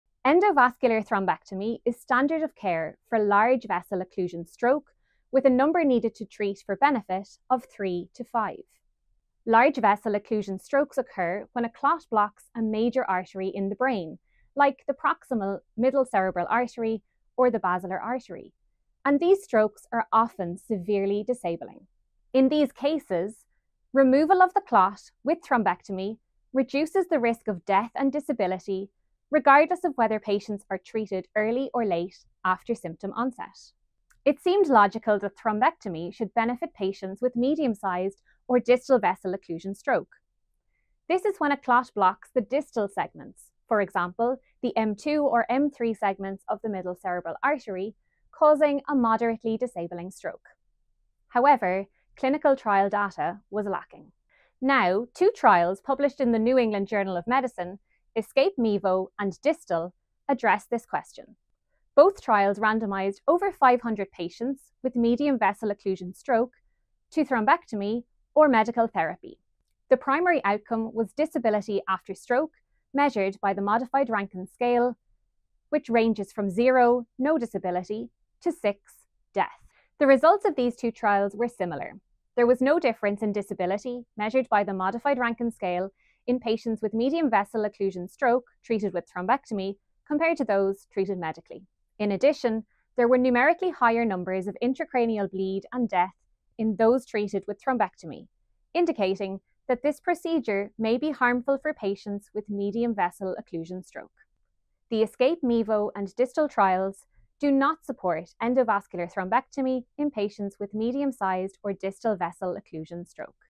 60-Second Journal Club: Sarah Gorey, MB, Summarizes the ESCAPE-MeVO and DISTAL Clinical Trials
Would thrombectomy benefit patients with medium-sized or distal vessel occlusion stroke? NEJM Editorial Fellow Sarah Gorey, MB, breaks down the ESCAPE-MeVO and DISTAL clinical trials.